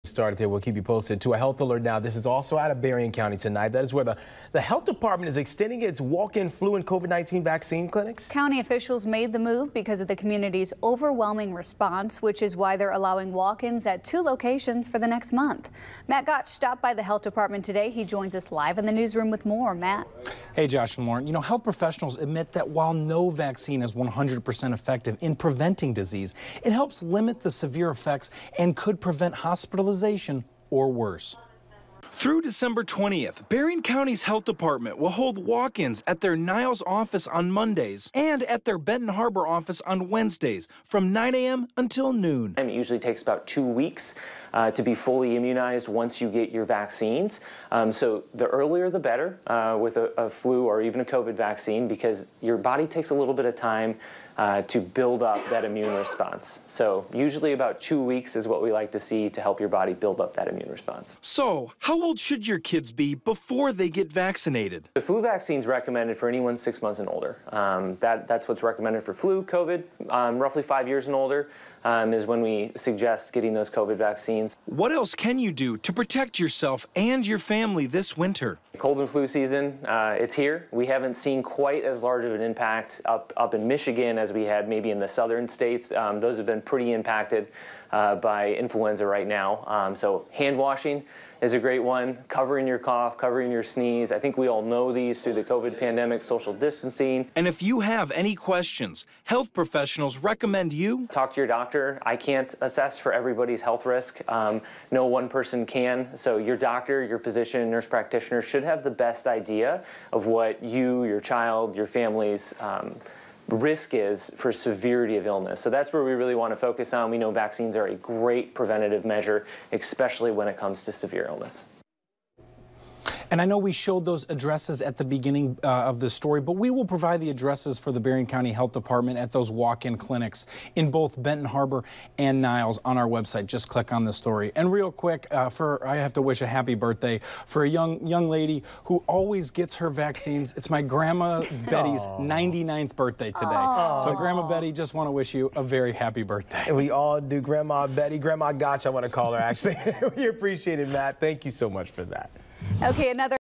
Berrien County Health Department extending COVID, flu vaccine clinics
Keep yourself healthy this winter by getting an updated flu shot.